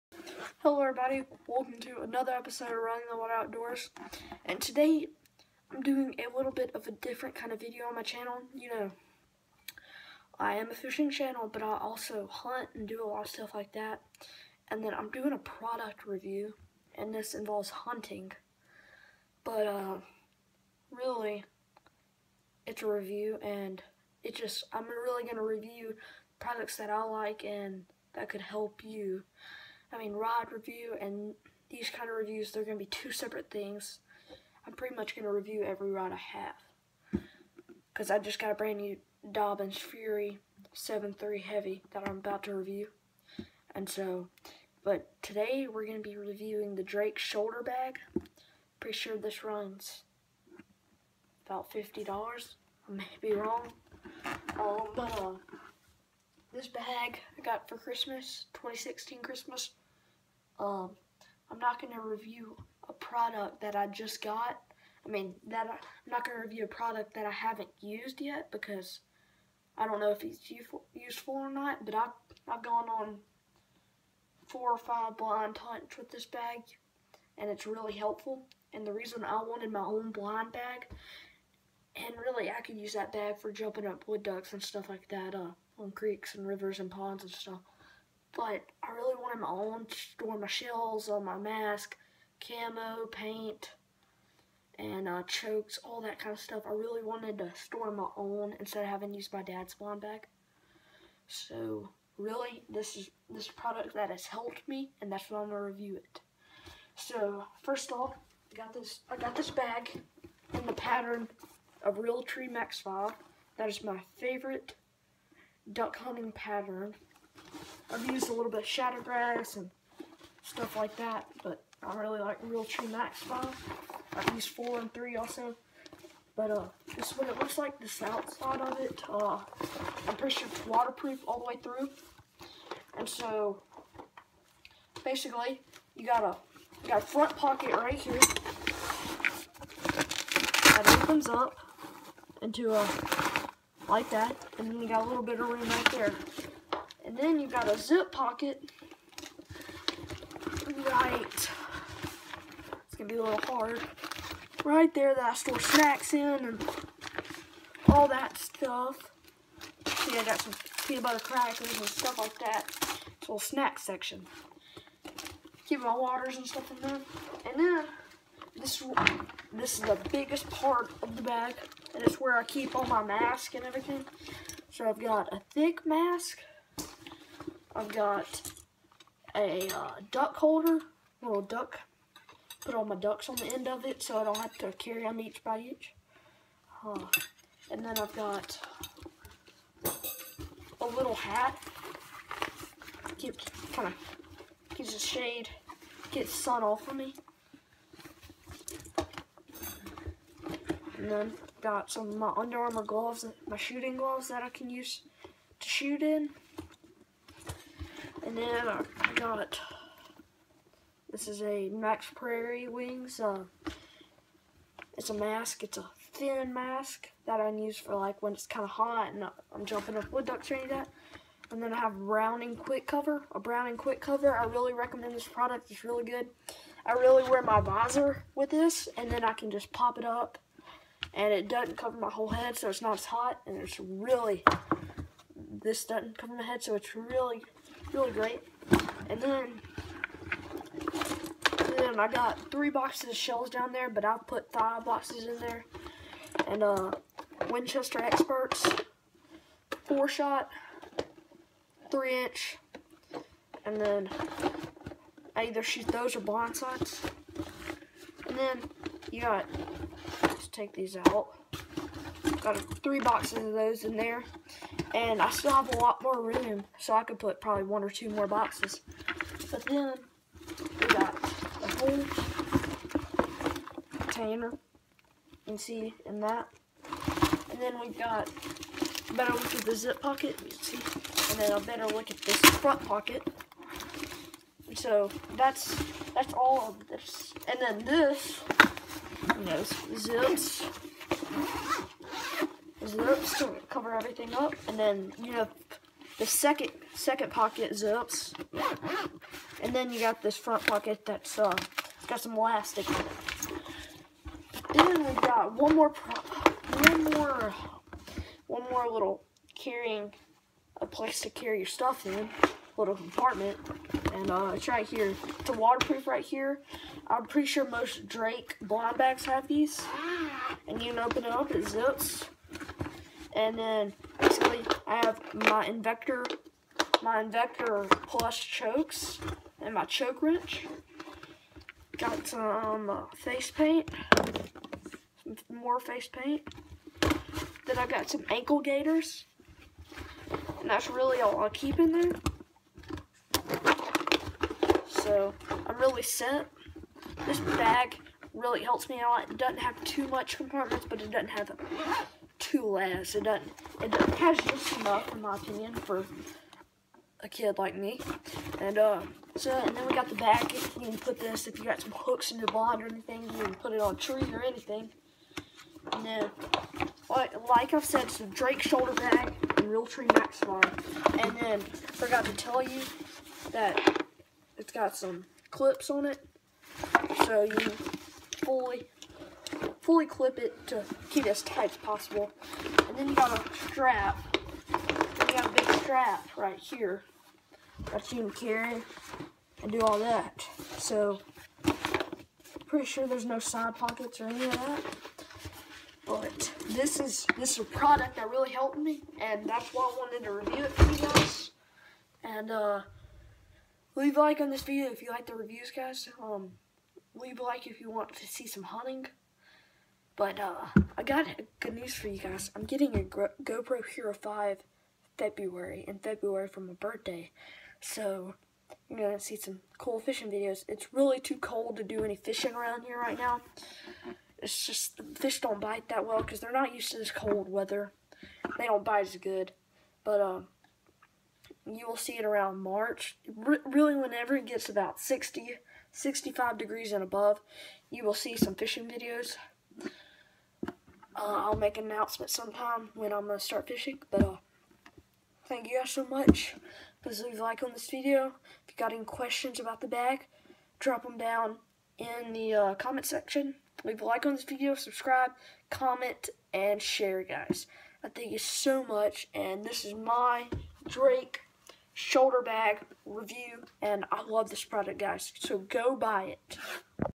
Drake Shoulder Bag Review
Great product! I also store my duck calls and beanie in this bag. Buy from anywhere that sells Drake products or Drake's website.